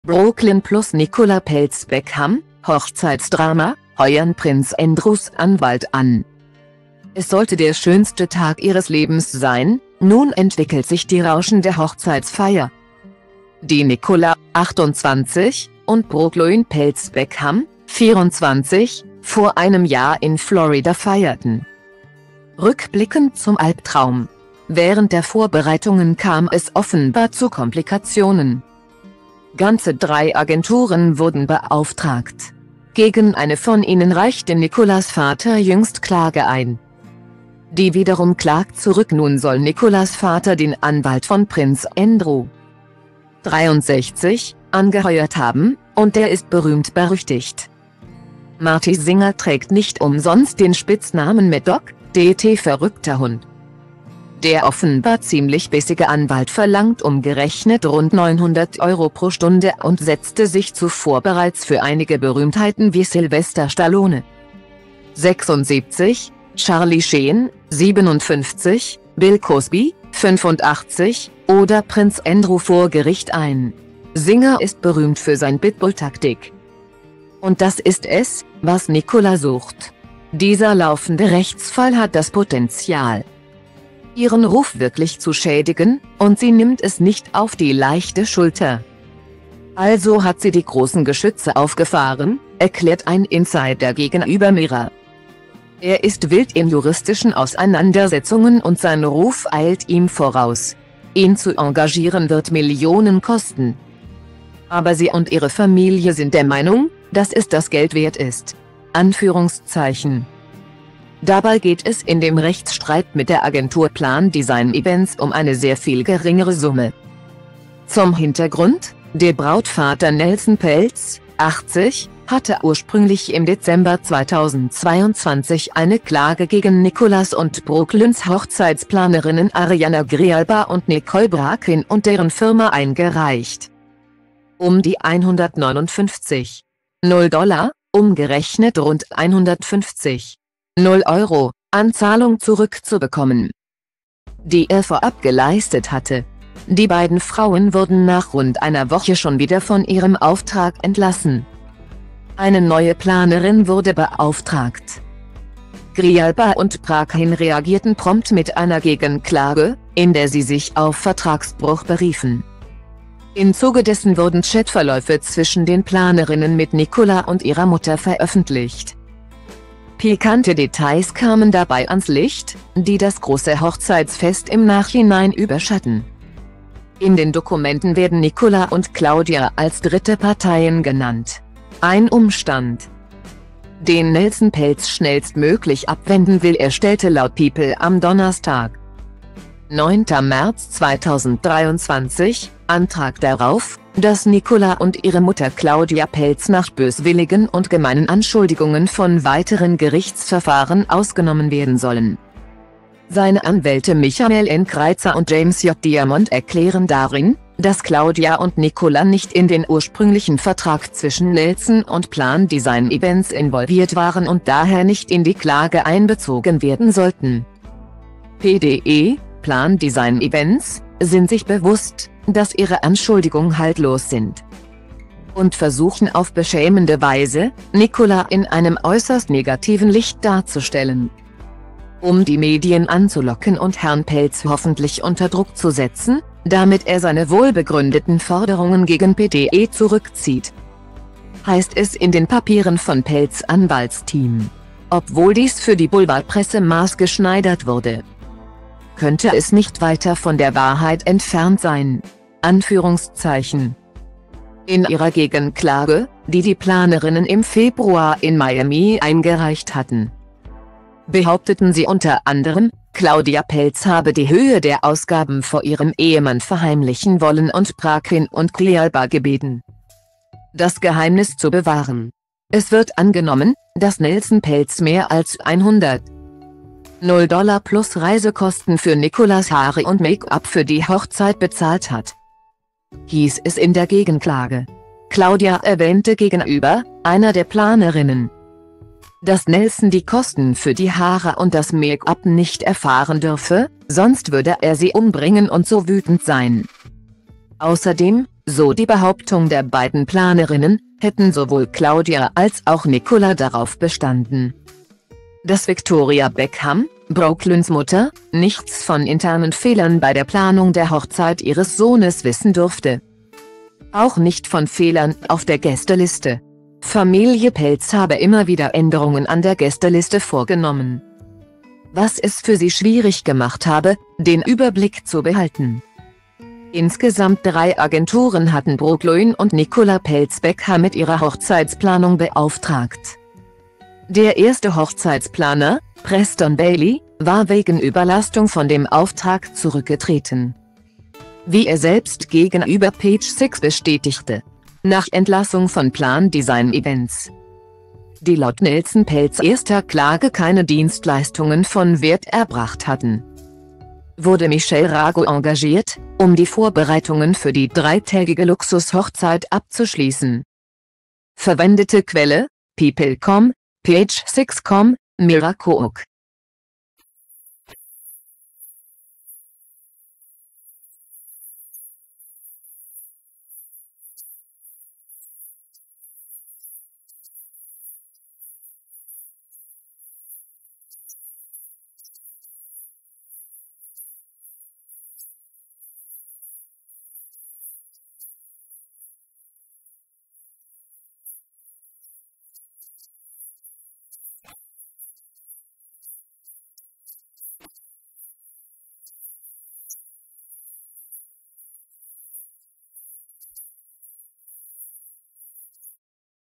Brooklyn + Nicola Peltz Beckham: Hochzeitsdrama! heuern Prinz Andrews Anwalt an
Es sollte der schönste Tag ihres Lebens sein, nun entwickelt sich die rauschende Hochzeitsfeier,die Nicola, 28, und Brooklyn Peltz Beckham, 24, vor einem Jahr in Florida feierten,rückblickend zum Albtraum. Während der Vorbereitungen kam es offenbar zu Komplikationen,